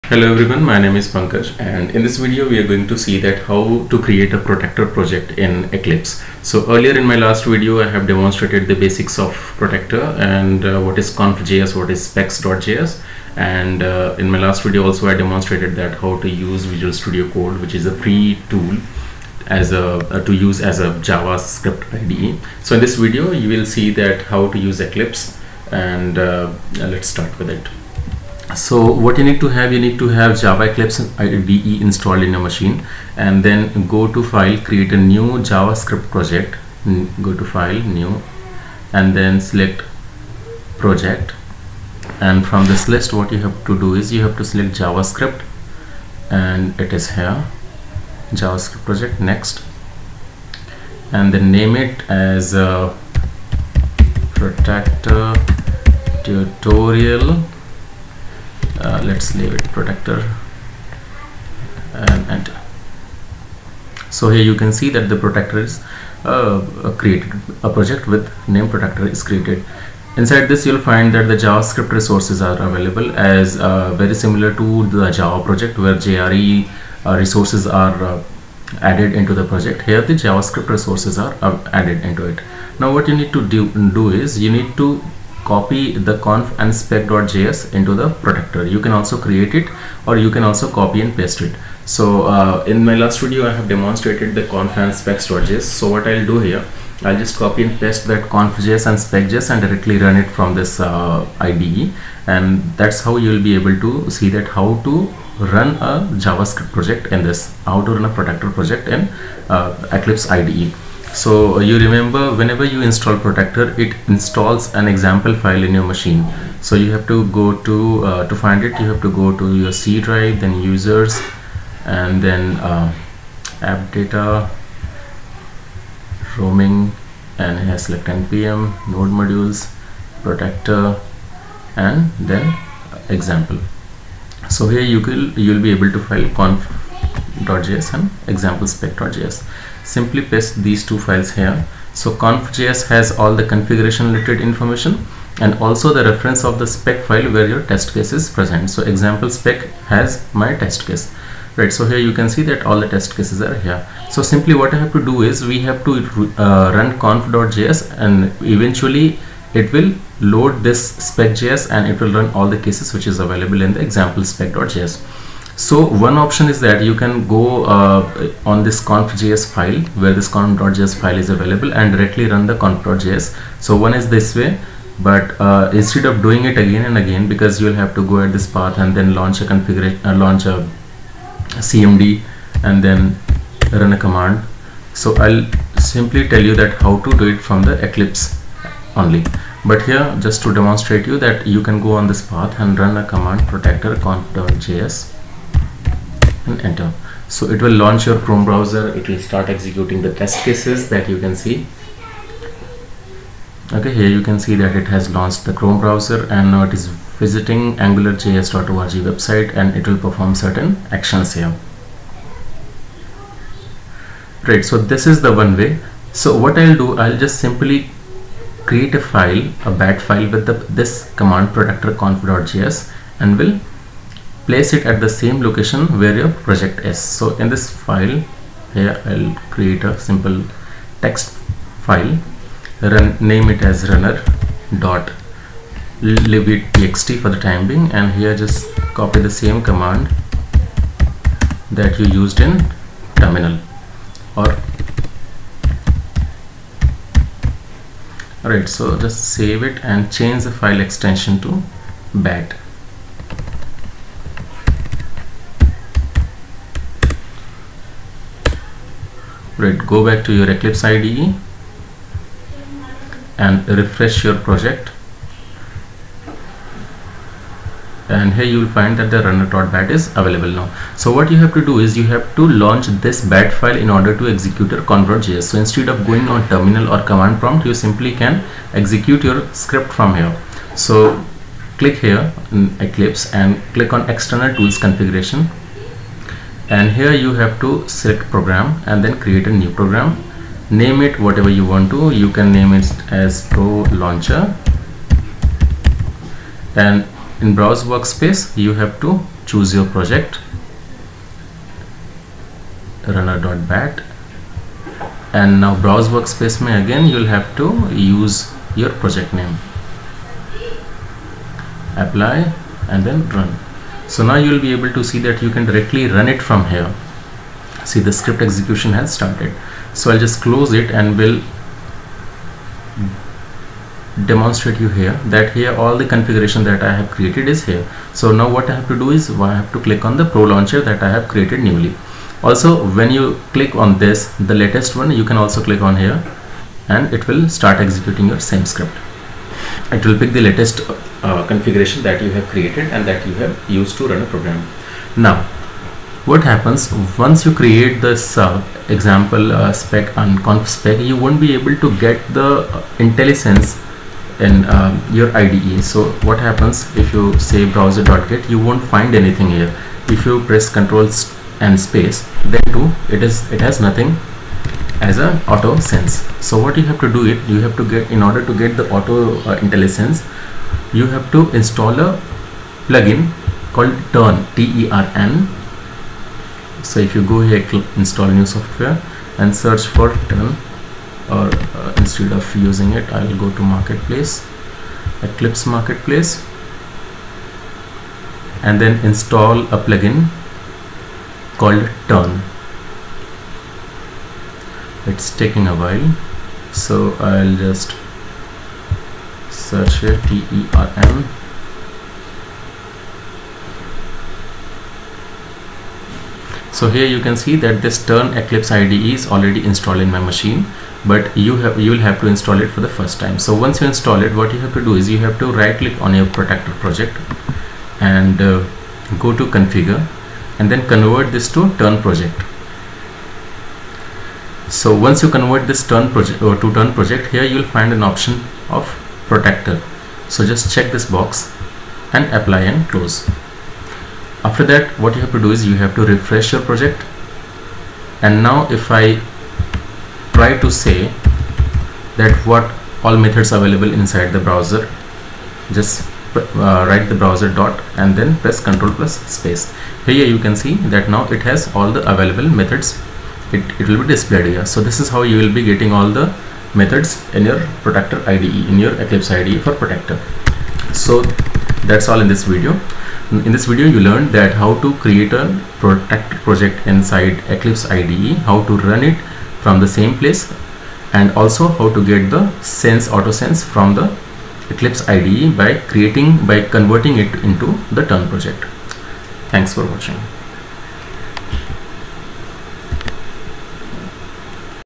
Protractor Setup in Eclipse with auto suggestions enabled
Learn how to create a protractor project in Eclipse IDE and also learn that how to enable auto sense in  Eciplse IDE for protractor.